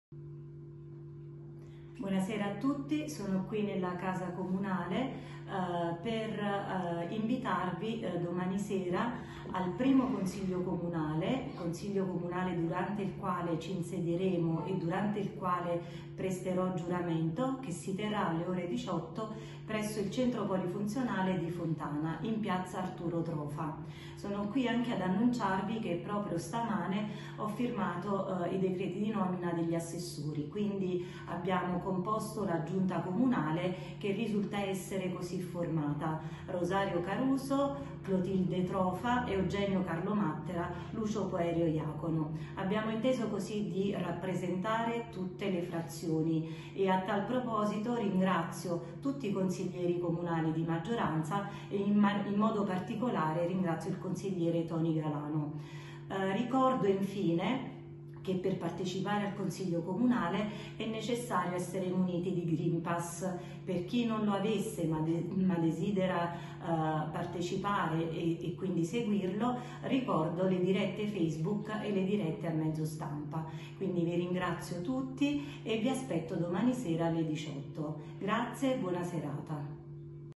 IRENE IACONO NOMINA LA SUA GIUNTA: CARUSO, TROFA, MATTERA E IACONO GLI ASSESSORI
SERRARA FONTANA - ALLA VIGILIA DEL CONSIGLIO COMUNALE DI INSEDIAMENTO IRENE IACONO NOMINA LA SUA GIUNTA: CARUSO, TROFA, MATTERA E IACONO GLI ASSESSORI

Il sindaco Irene Iacono ha nominato nella tarda mattinata di oggi  i quattro assessori che compongono la giunta, affidando anche l’incarico di vice sindaco.
Gli assessori sono:
Rosario Caruso, ex sindaco, (sarà anche vice vice sindaco);
Tilde Trofa;
Eugenio Carlo Mattera;
Lucio Poerio Iacono.
Le deleghe saranno assegnate con successivo provvedimento.

Domani alle 18:00 il consiglio comunale di insediamento (in diretta su Nuvola Tv).